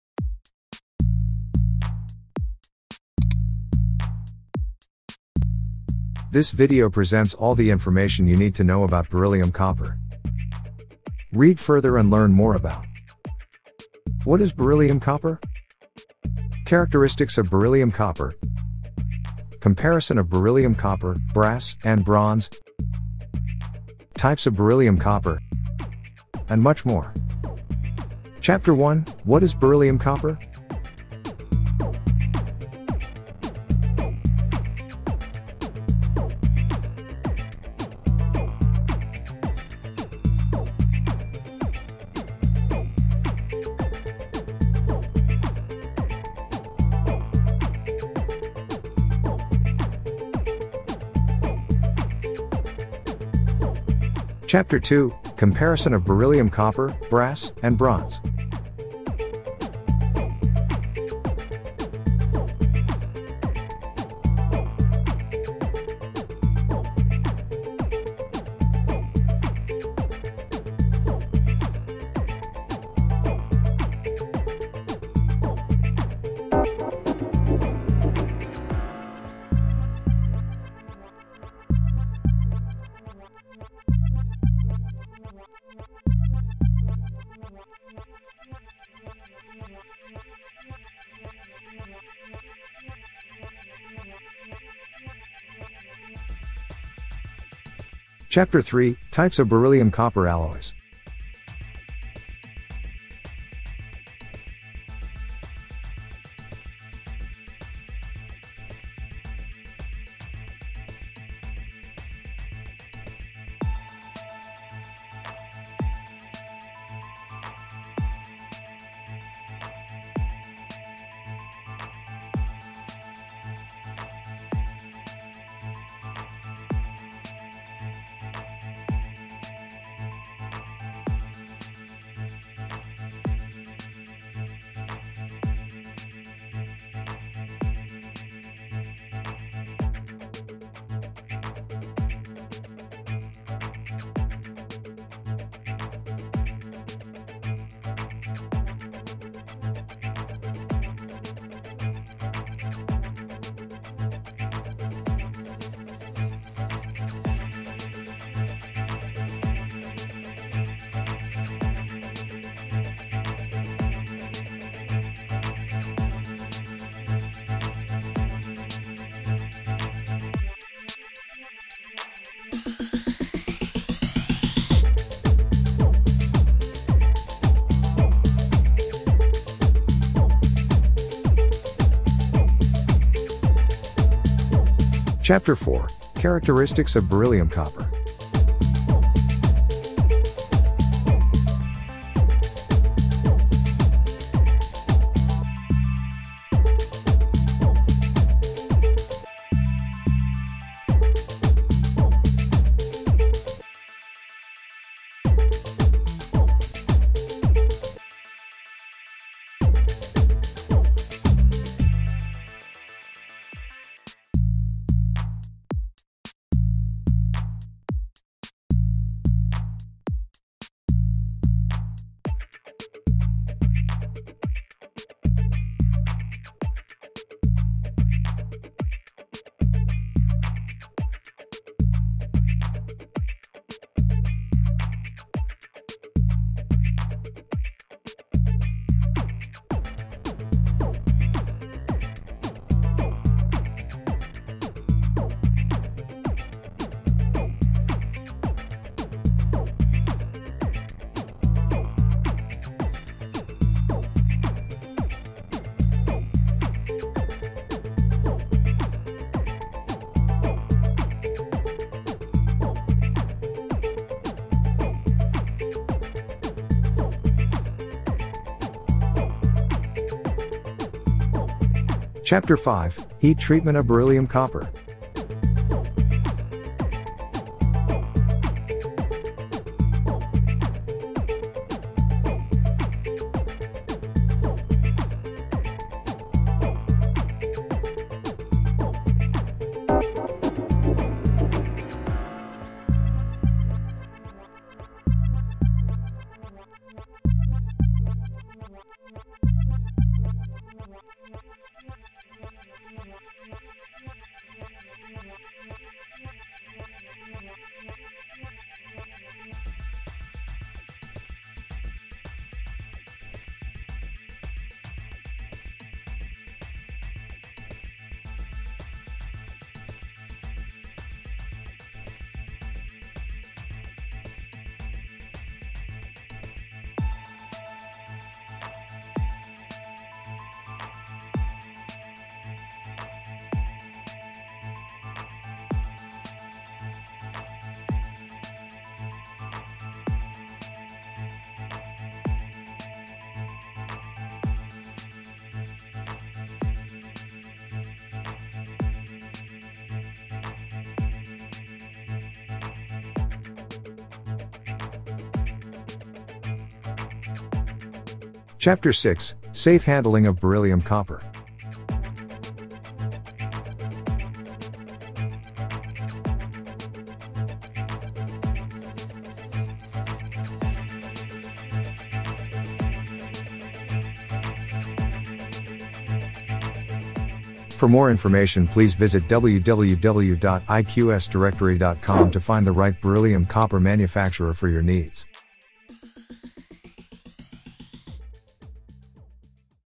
Beryllium Copper: The Complete Guide (2021 Update)
Our updated Beryllium Copper guide for 2021! In our all-new video, we discuss topics such as: What beryllium copper is, beryllium copper characteristics, comparisons of beryllium copper, brass, & bronze, beryllium copper types, and much more…